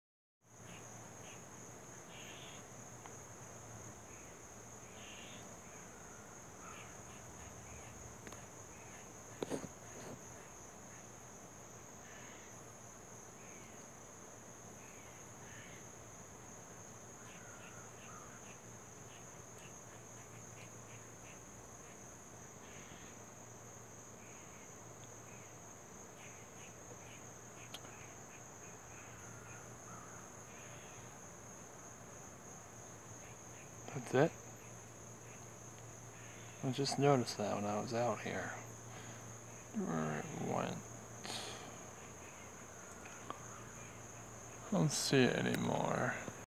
Stray Cat in Back Yard
What do you guys think? Is it just a stray or is it someone else's wondering?